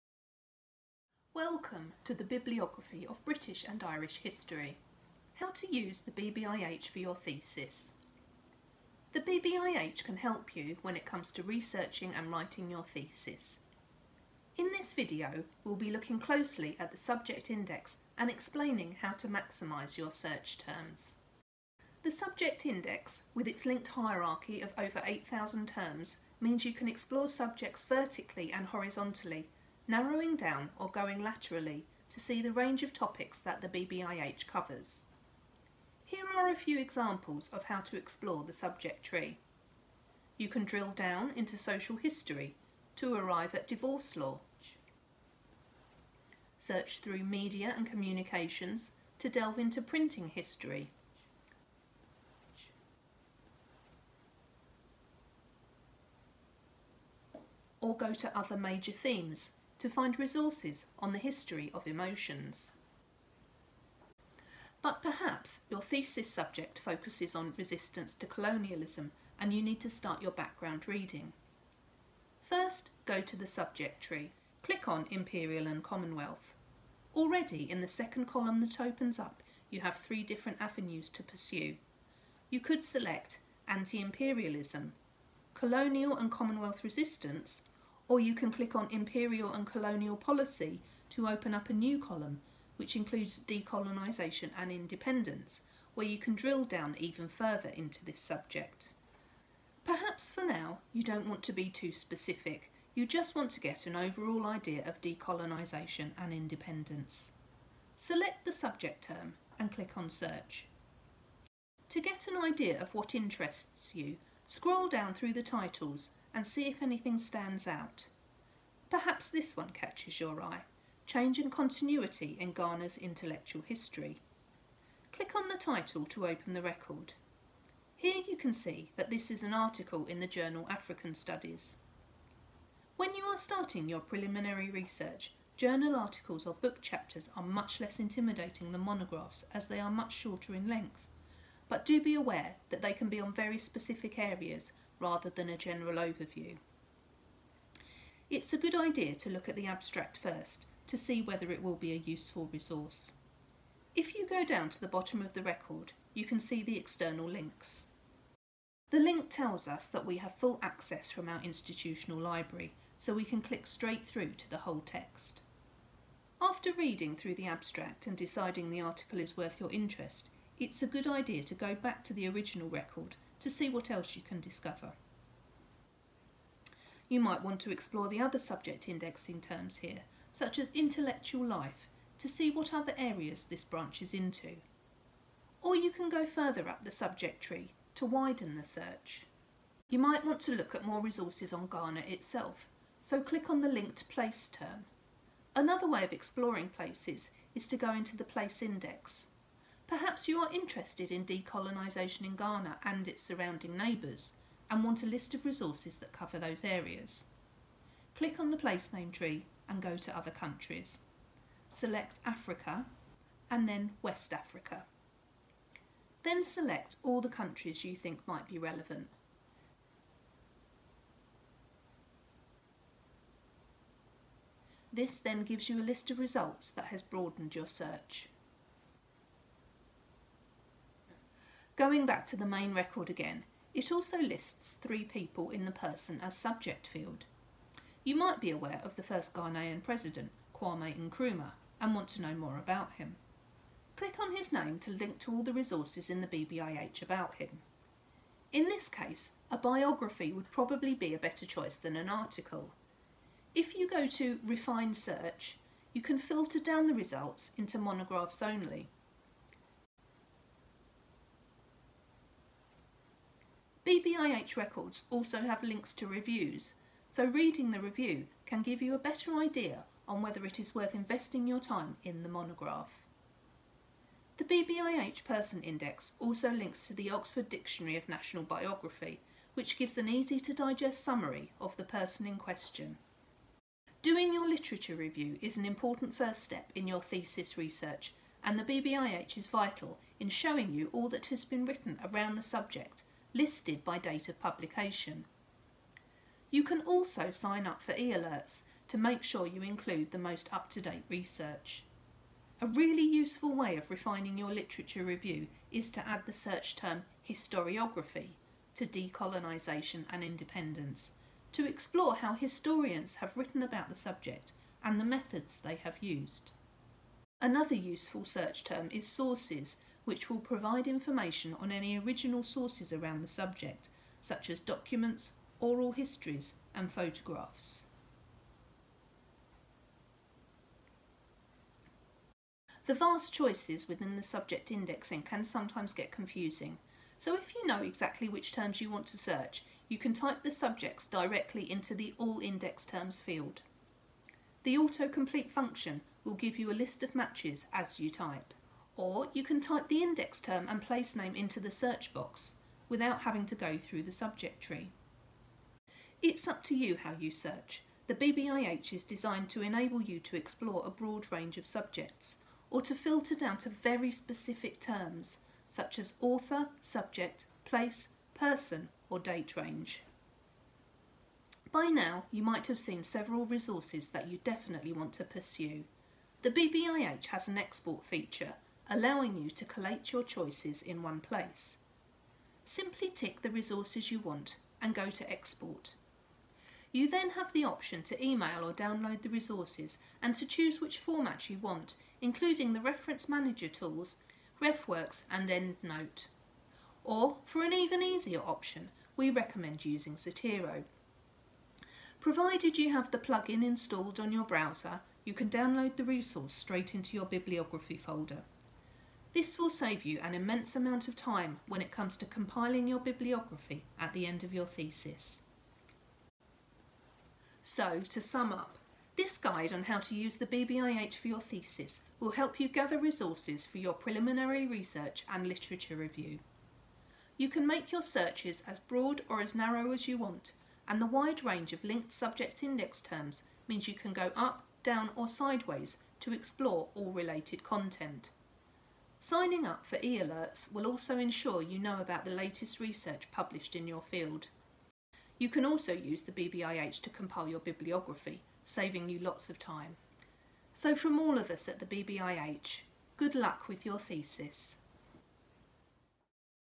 How to use BBIH for your MA or PhD thesis
In this video we explore how BBIH can help when researching and writing your MA or PhD thesis. The Bibliography's 625,000 records cover all aspects of British and Irish History, within the British Isles and worldwide. This makes BBIH the essential resource for understanding the historiography of your thesis topic and in preparing your all important literature review.

The Bibliography will also provide you with detailed information on the latest publications in your primary area of research, as well provide all you need on publications in secondary fields that might be considered in specific chapters of your dissertation. This video shows you how to search BBIH using its rich metadata and indexing to ensure you identify all relevant material for your research.

Video chapters:
0:00 Introduction
0:22 Exploring the BBIH subject tree
1:52 Exploring the BBIH results page
2:07 Exploring a BBIH record
3:10 Exploring the BBIH place name tree
4:23 Exploring reviews in BBIH
4:33 Exploring people as subjects in BBIH
4:44 Using BBIH when preparing your literature review
6:12 Exporting references from BBIH
6:38 Using Zotero with BBIH
6:57 Summary

Next video in the series:
Behind the scenes at BBIH